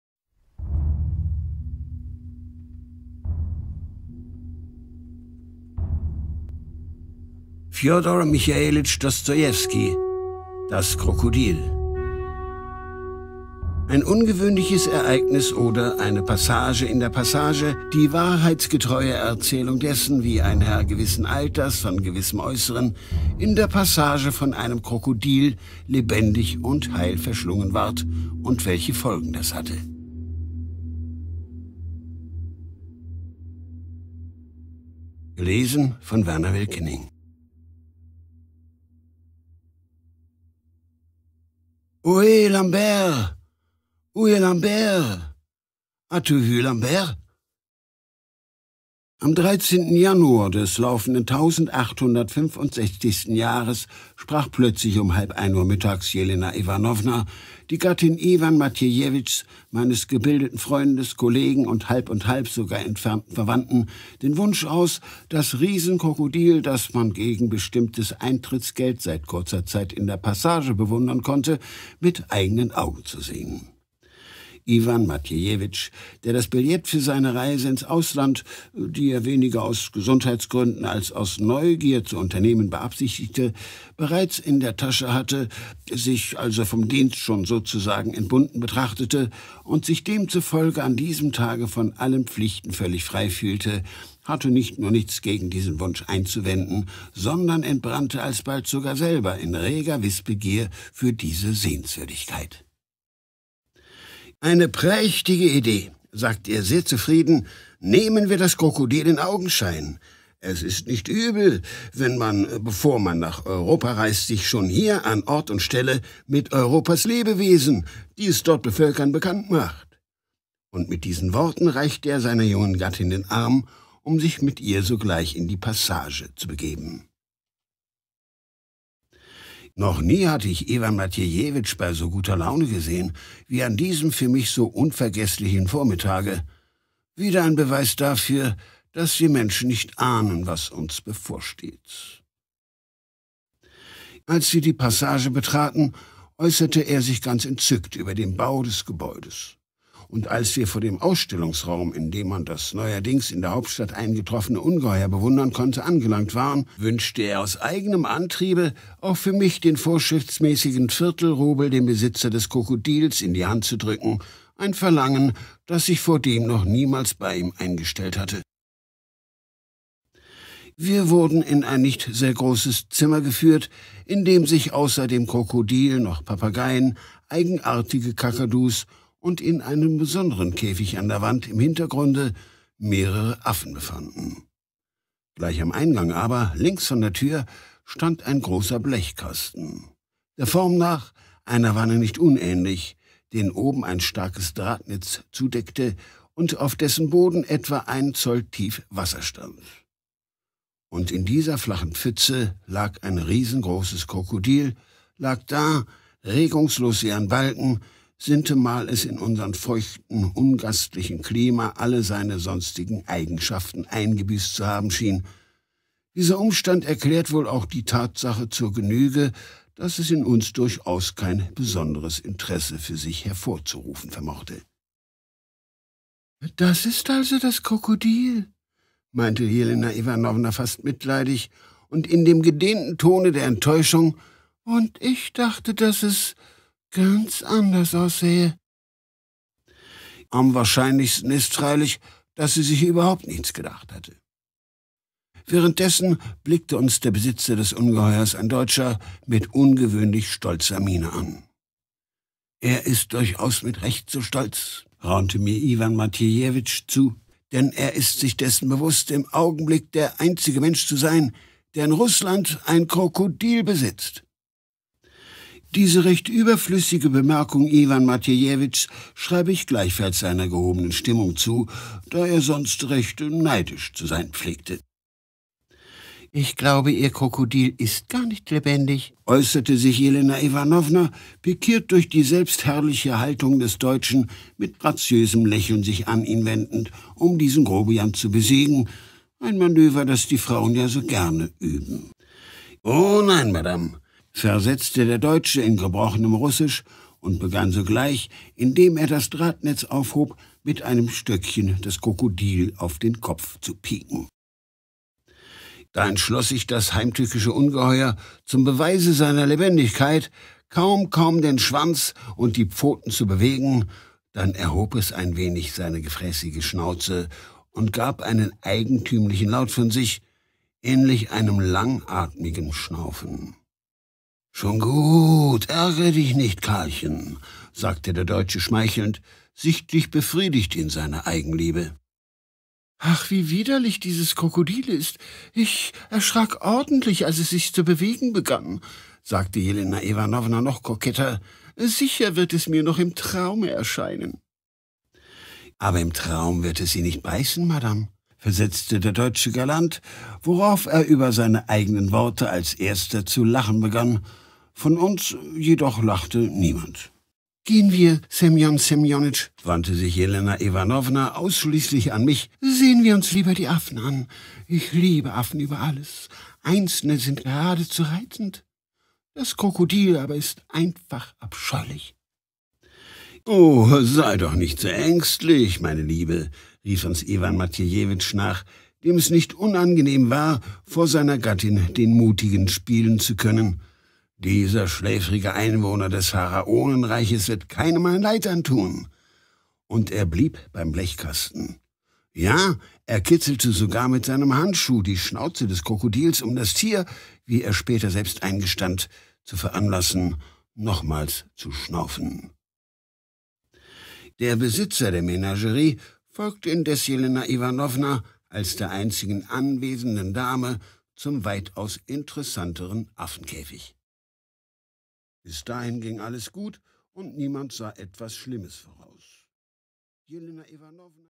Fjodor Dostojewski : Das Krokodil (Hörprobe)
Eine Groteske des Meisters, die in ihrer Absurdität an Gogols "Die Nase" erinnert.
Übertragung E. T- Rahsin (1920)
Sprecher : Werner Wilkening
Erschienen bei words & music